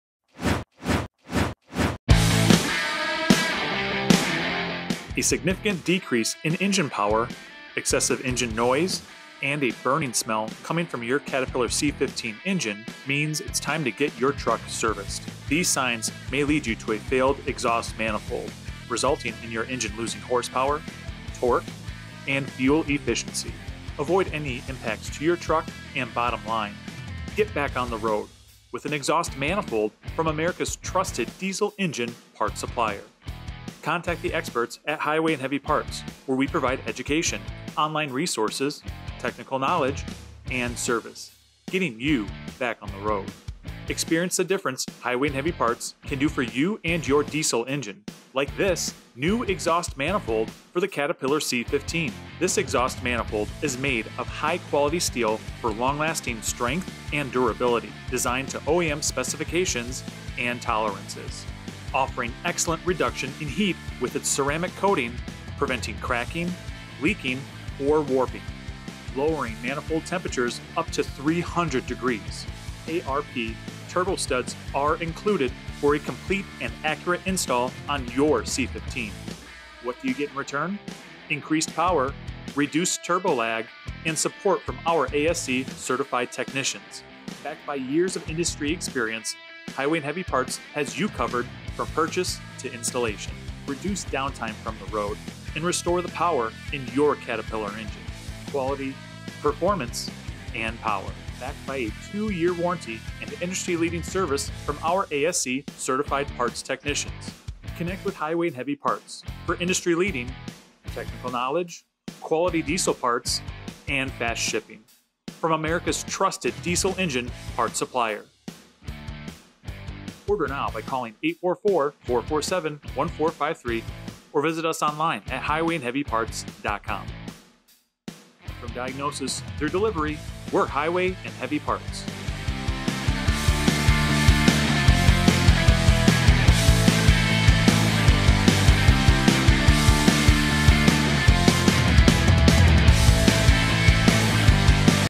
Caterpillar C15 Exhaust Manifolds From HHP. Check This Out!
Restore Power with a New Caterpillar C15 Exhaust Manifold

A significant decrease in engine power, excessive noise, and a burning smell from your Caterpillar C15 engine signal it's time for service. These signs often point to a failed exhaust manifold, resulting in lost horsepower, torque, and fuel efficiency. Avoid these issues and get back on the road with a new exhaust manifold from America's trusted diesel engine parts supplier, Highway and Heavy Parts.

Step-by-Step Guide/Tutorial:
Identify Symptoms: Look for decreased engine power, excessive noise, and burning smells.
Inspect Exhaust Manifold: Check for cracks, leaks, or warping in the manifold.
Choose Quality Replacement: Select a high-quality exhaust manifold from Highway and Heavy Parts.
Install Manifold: Follow OEM specifications and include ARP turbo studs for accuracy.
Test Performance: Ensure increased power and reduced turbo lag after installation.

What You Will Learn:
How to identify signs of a failing exhaust manifold.
The benefits of using a high-quality steel exhaust manifold.
The importance of ceramic coating for heat reduction.
Steps for proper installation of an exhaust manifold.
How a new manifold improves engine performance and efficiency.

Key Benefits:
Increased Power: Restores lost horsepower and torque.
Reduced Turbo Lag: Improves engine response time.
Durability: High-quality steel and ceramic coating prevent cracking and warping.
Expert Support: Access to ASE-certified technicians for guidance.
Peace of Mind: Backed by a two-year warranty and fast shipping.